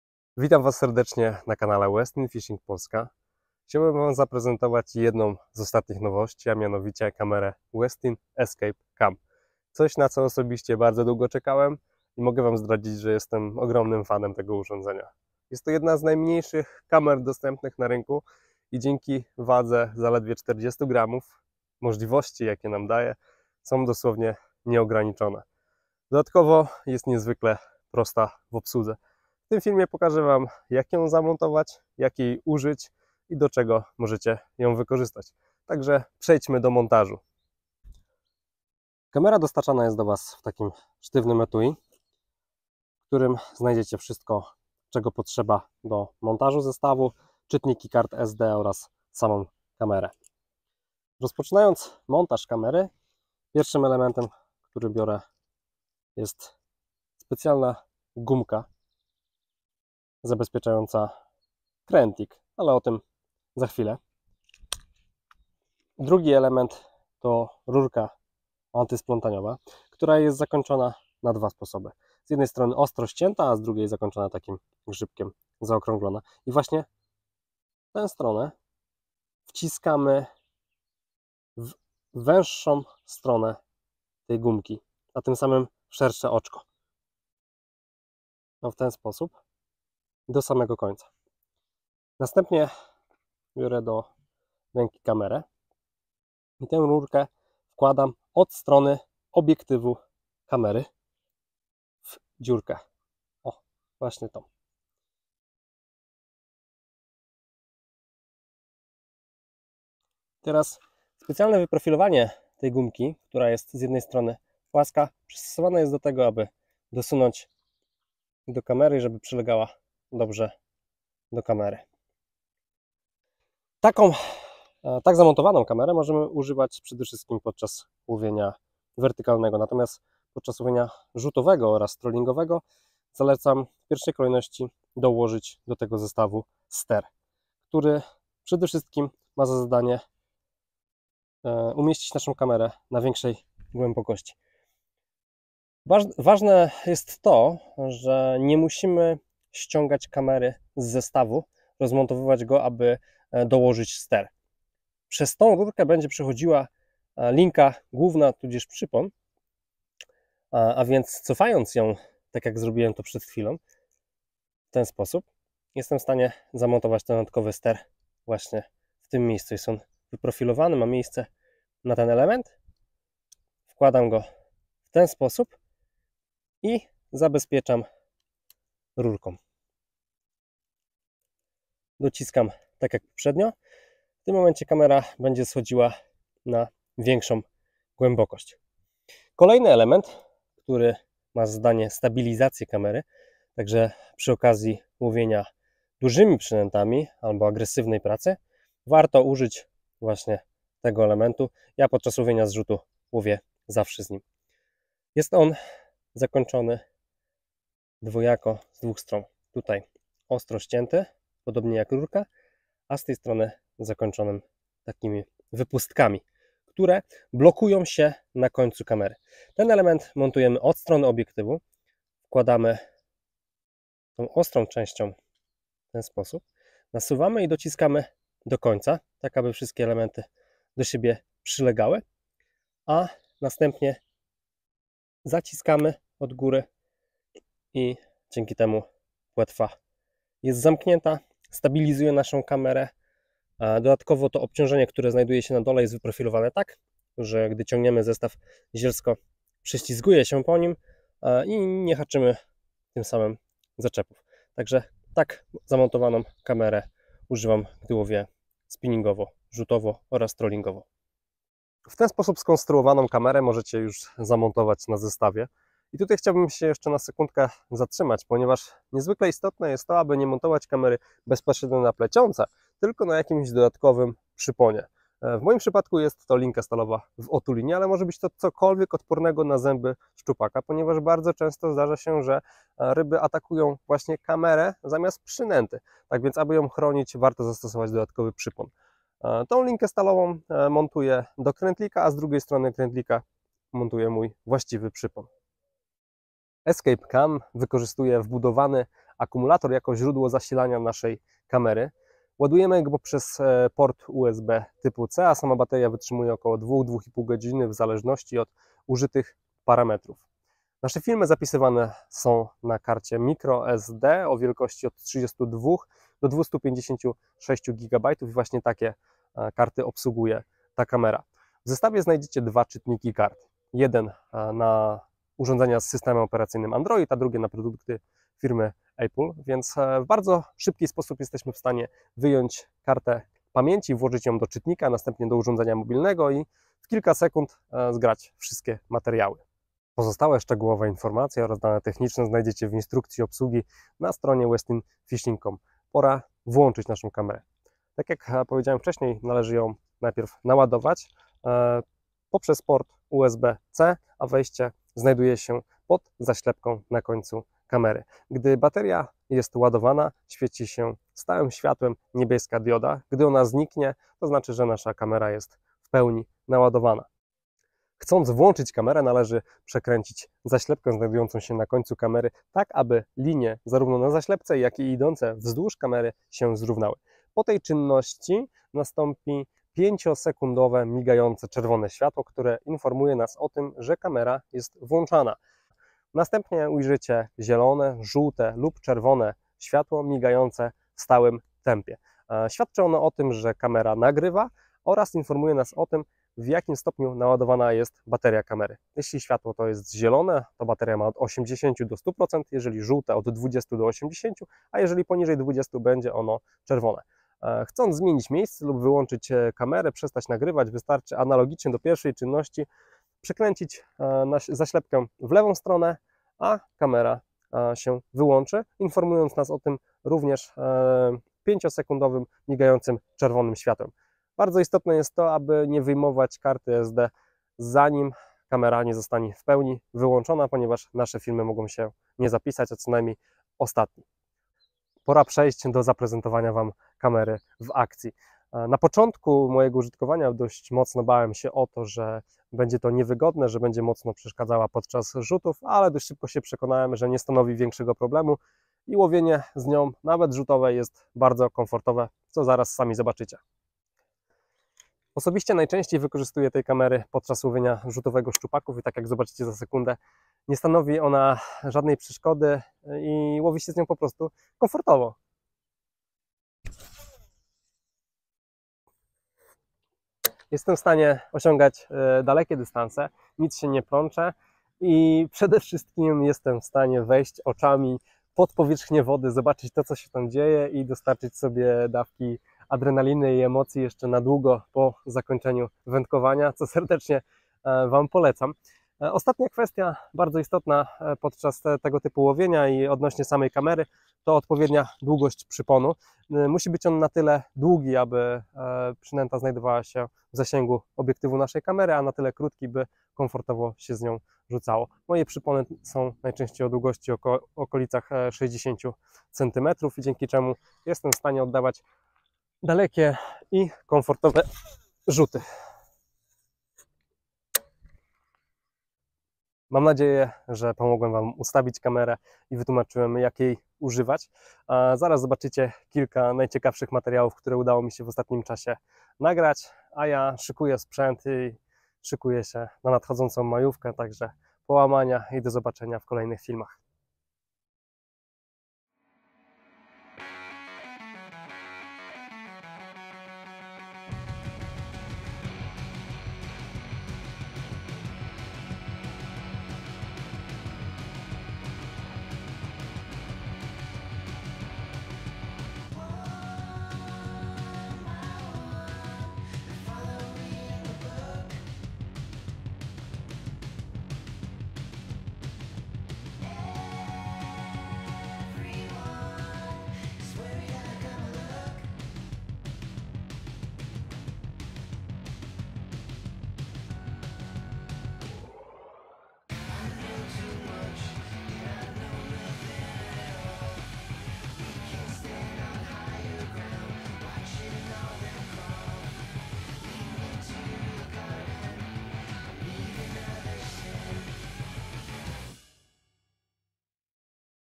Westin Escape Cam - Obsługa Kamery I Jak montować kamerę
Kacper Dowksza w filmie tym pokazuje, jak prawidłowo montować kamerę wędkarską Westin Escape Cam do swojego zestawu oraz przybliża jej obsługę.

Oglądaj koniecznie do końca, aby zobaczyć Escape Cam w akcji oraz to, co dzięki niej nagrał Kacper podczas łowienia.